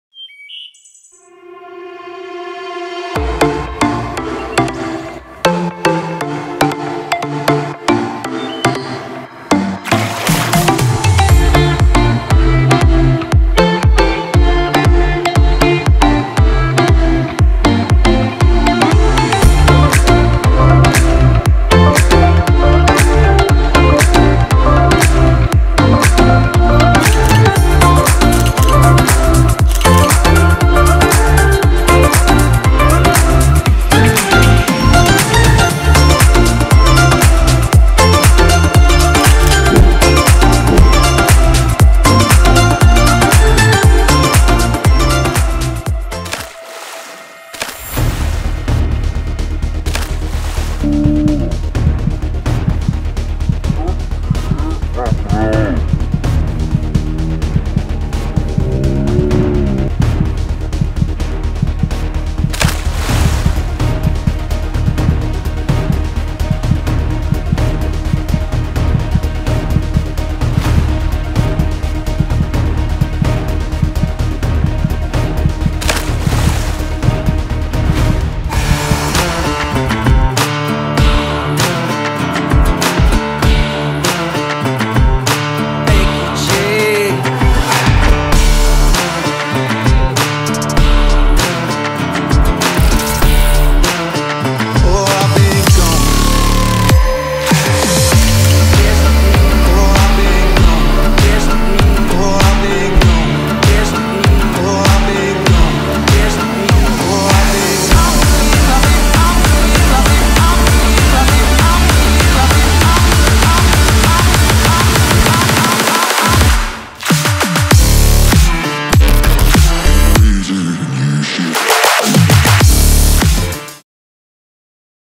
Chèo Sup ở Đà Lạt - Galaxy Z Flip3 rơi xuống hồ Tuyền Lâm sẽ thế nào?
Clip vui team quay cùng Z Flip3 với một vài trải nghiệm ở Đà Lạt - Hi vọng các bạn sẽ thấy thích video này!